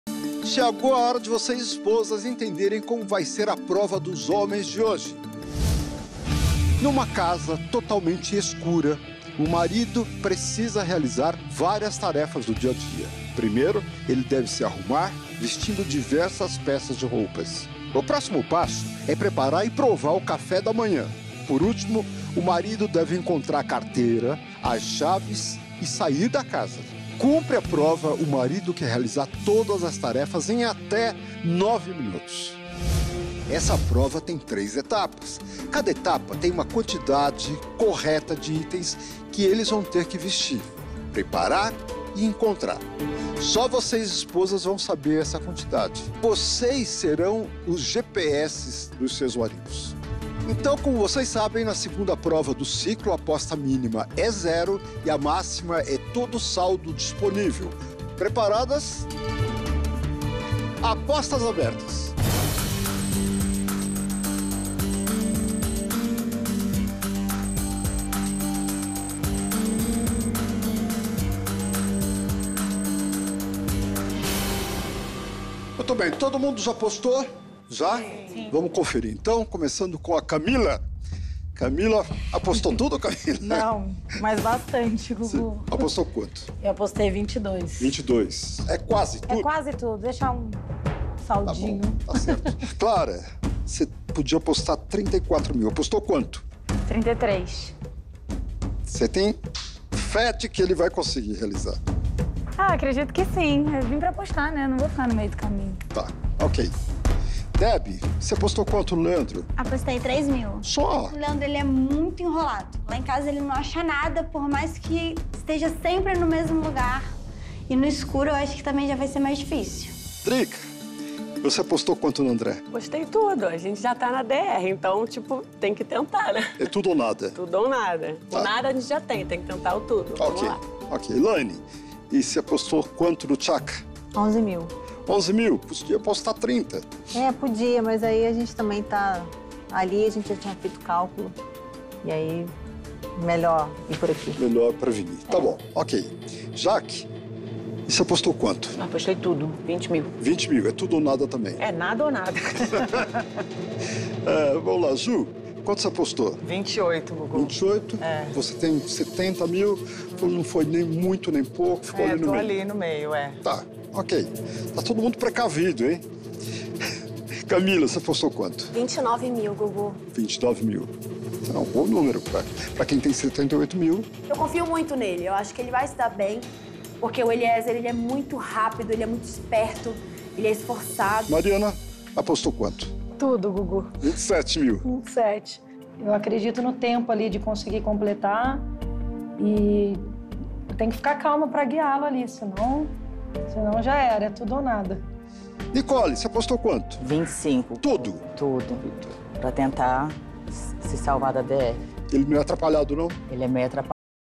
Clara Maia, Kamilla Salgado e Ju Valcézia fazem as apostas mais altas
Gugu Liberato se reuniu com as mulheres na Sala de Apostas, onde ele explicou as regras da Prova dos Homens da semana.

Depois de assistirem ao vídeo com as instruções, elas fizeram as apostas nos maridos, confinando na capacidade deles para o desafio: fazer uma sequência de tarefas no escuro.

Clara Maia, Kamilla Salgado e Ju Valcézia foram as participantes que fizeram as apostas mais ousadas: R$ 33, R$ 29 e R$ 28 mil, respectivamente.